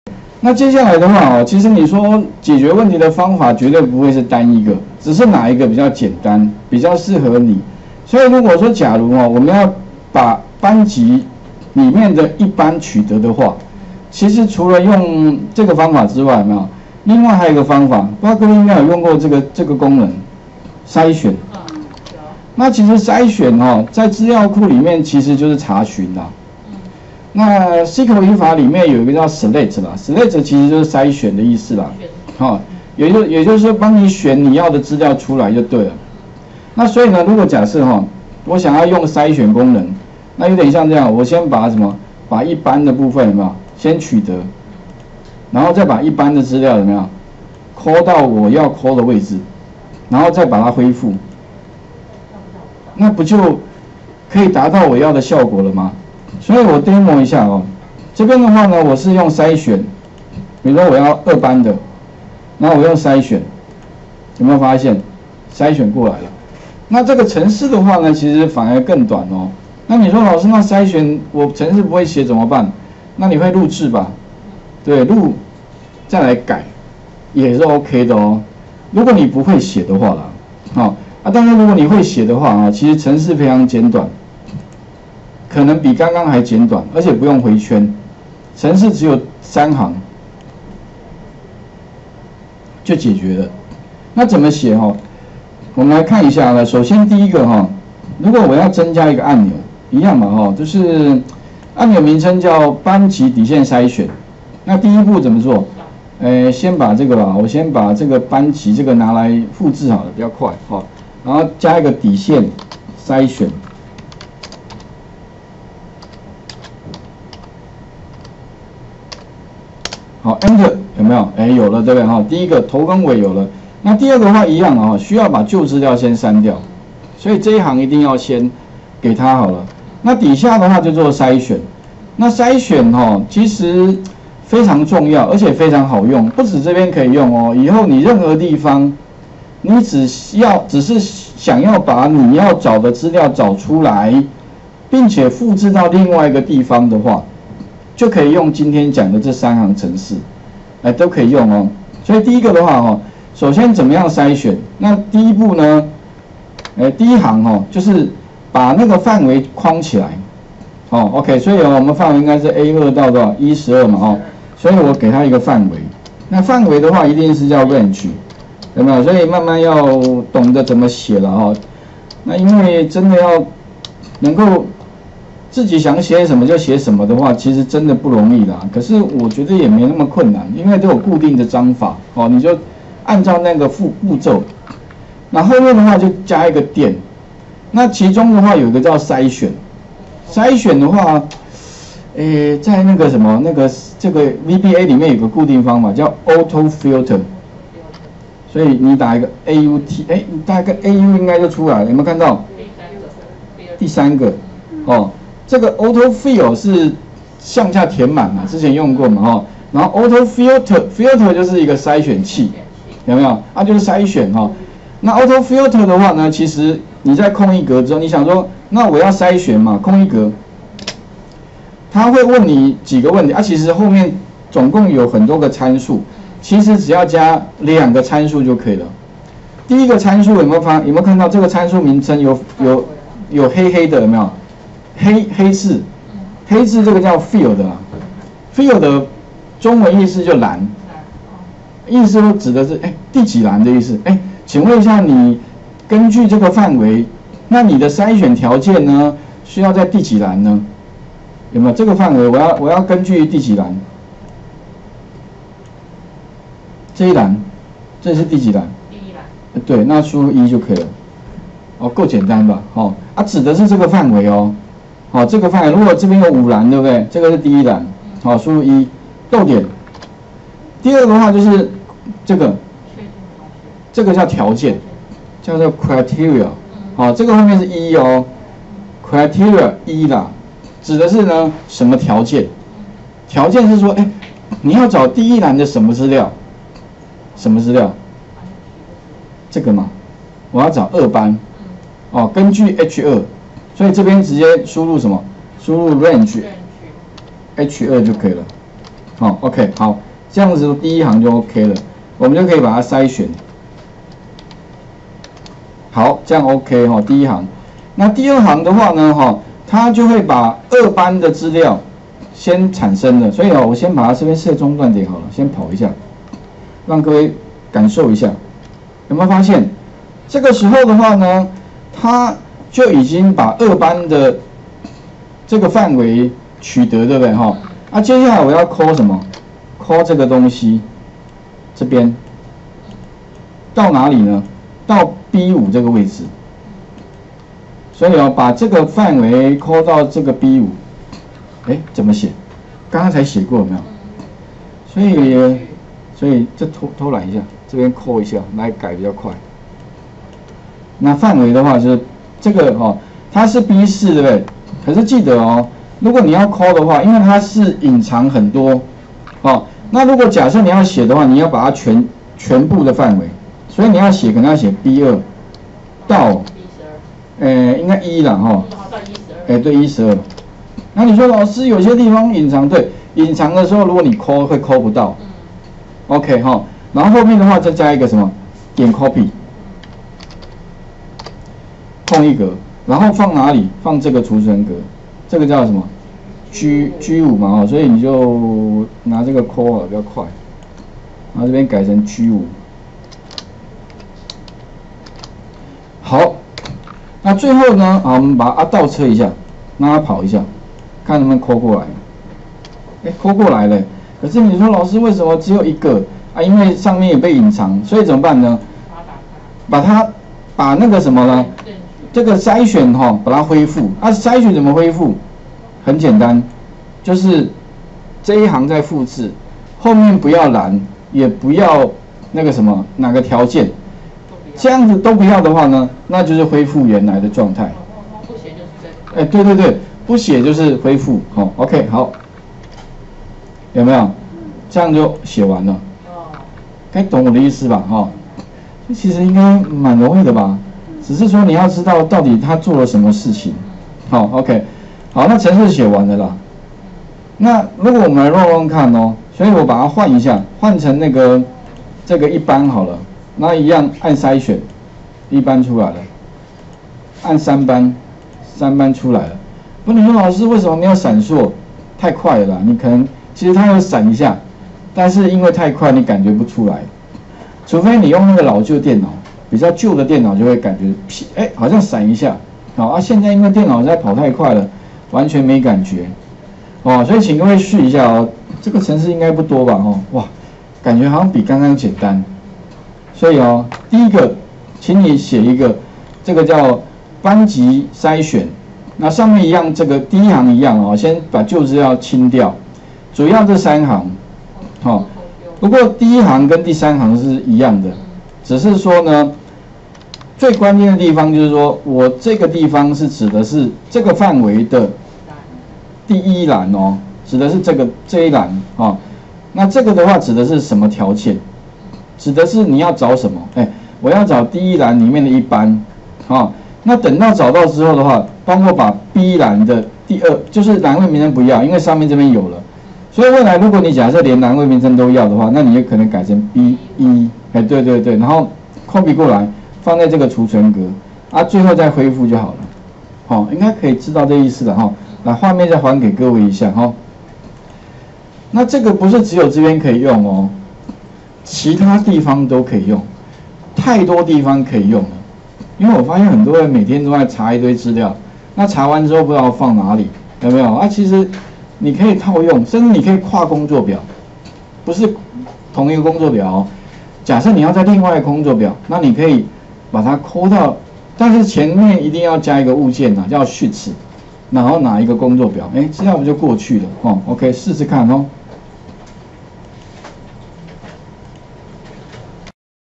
02 將班級改為篩選AutoFilter方式
從EXCEL高階函數與VBA設計第14次上課

上課內容：
01_重點複習與326班級VBA改為Range物件
02_將班級改為篩選AutoFilter方式
03_改為事件驅動H2儲存格
04_樞紐分析表錄製巨集與自動增加列與欄
05_樞紐分喜表VBA與結尾

上課用書：
Excel VBA一點都不難：一鍵搞定所有報表
作者： Excel Home
出版社：博碩
出版日期：2013/06/26
定價：380元

吳老師  112/10/21